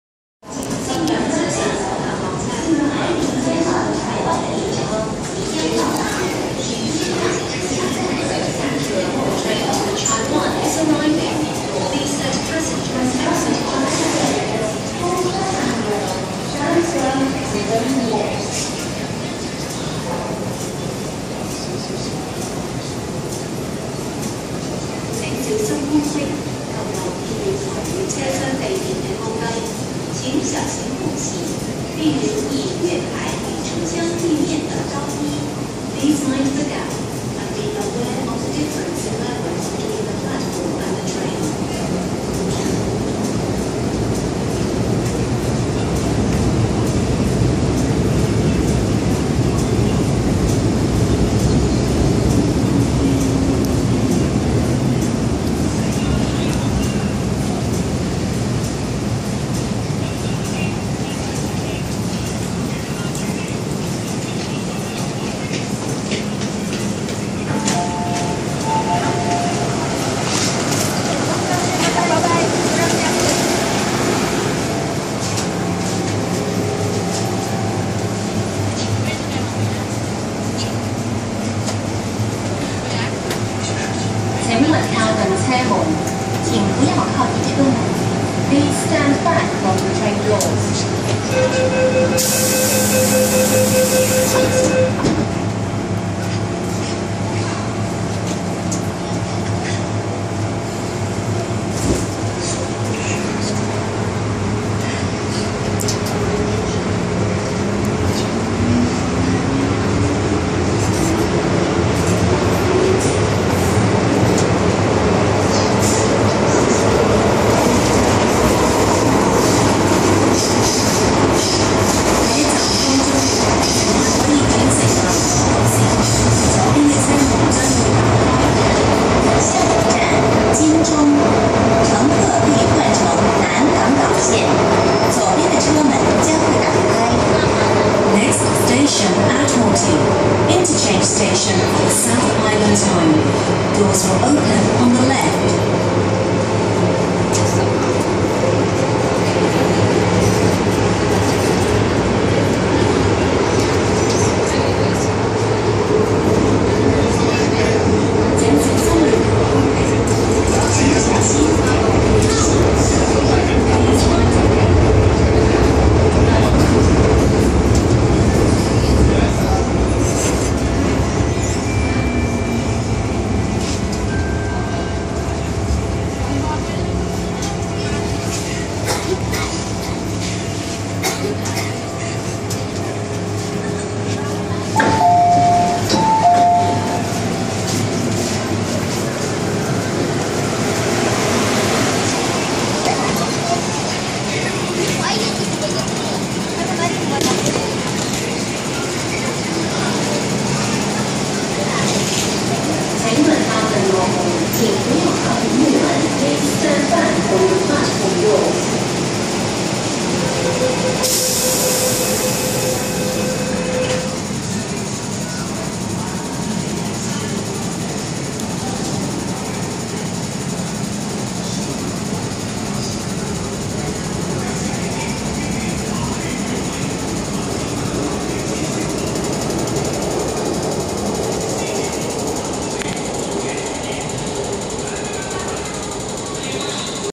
港島綫 M-Train A202/A253 (C253) CEN → ADM
拍攝日期: 14/7/2019 (日)
列車：港島綫 A202/A253 (C253)，車序：013

A202/A253今日下繁喺TKL行013。